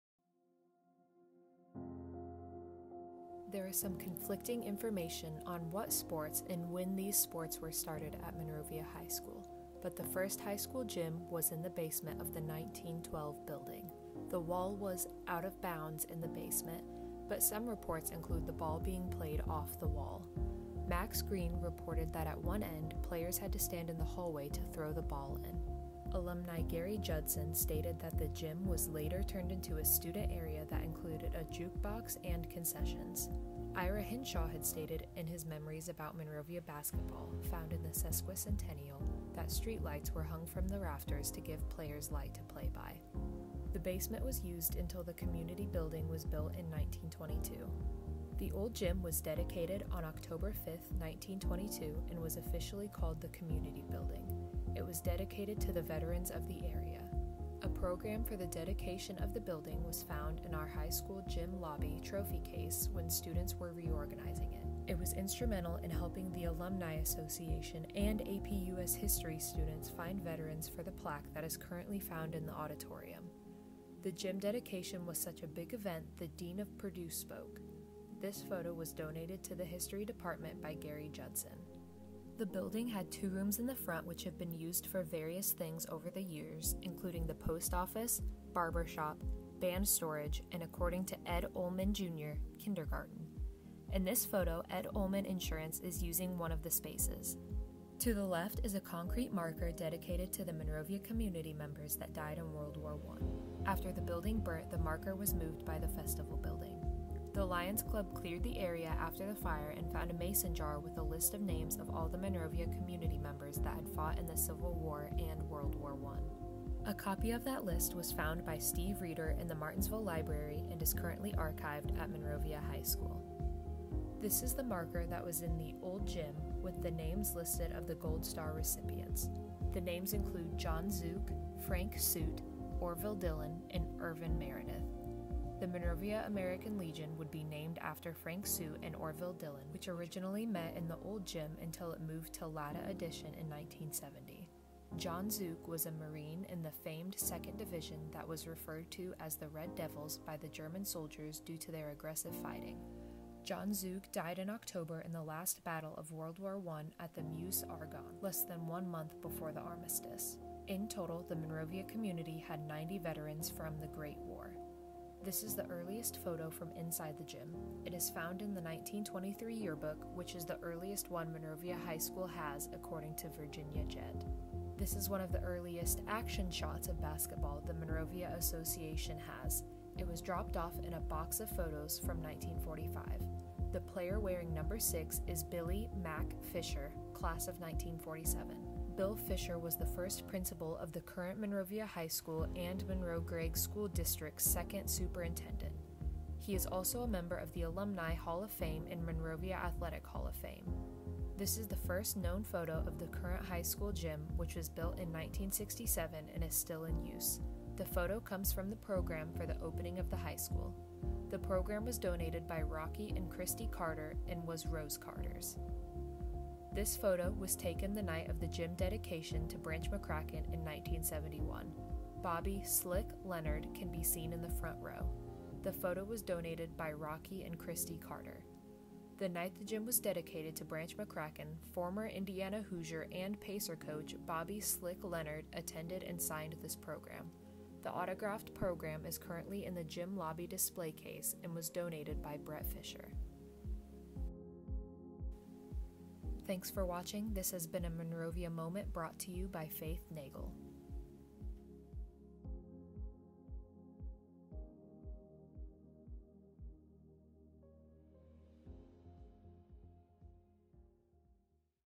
Monrovia Moments- The Old Gym
This video is about the small town of Monrovia, Indiana and the history of its old gym and the different buildings it has gone through throughout the area.